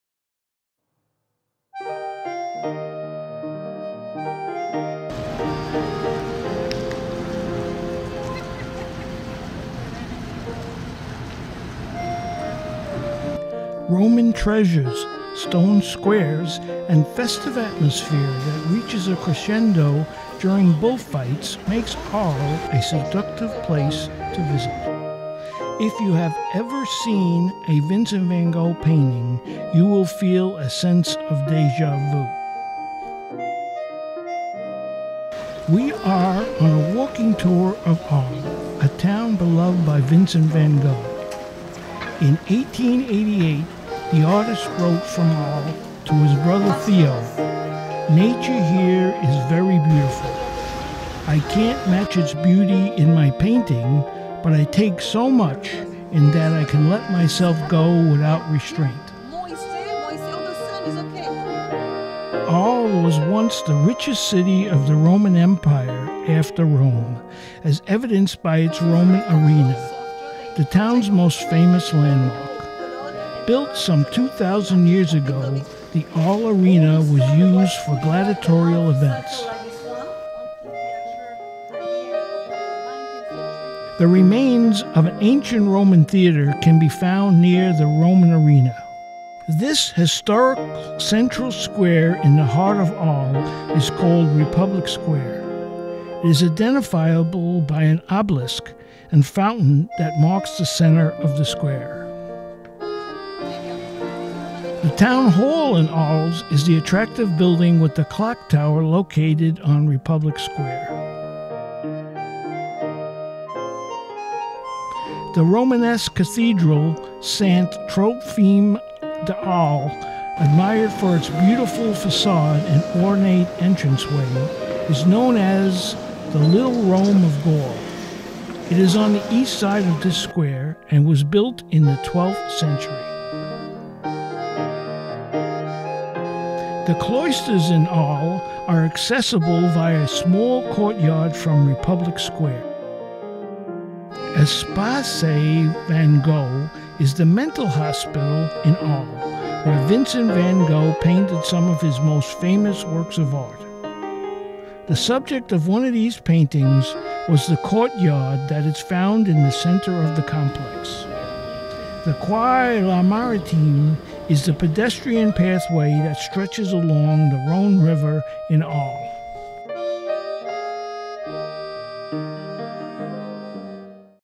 Arles, France
Arles is a city on the Rhône River in the Provence region of southern France. It's famed for inspiring the paintings of Van Gogh.  Once a provincial capital of ancient Rome, Arles is also known for many remains from that era, including Arles Amphitheatre, now hosting plays, concerts and bullfights.